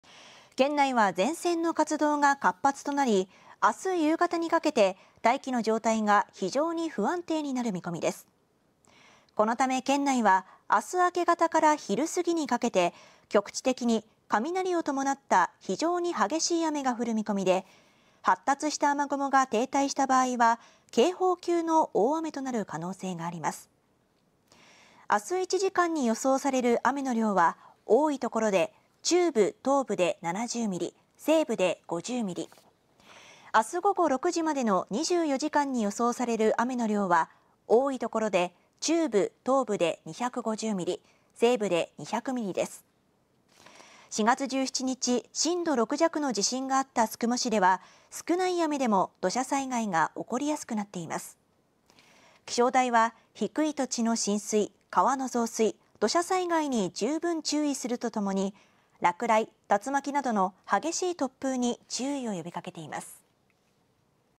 「28日明け方から高知県内で警報級の大雨となる可能性」
高知県内は28日明け方から昼過ぎにかけて局地的に雷を伴い、多い所で1時間に70ミリの非常に激しい雨が降る見込み。気象台は低い土地の浸水、川の増水、土砂災害に十分注意するよう呼びかけている。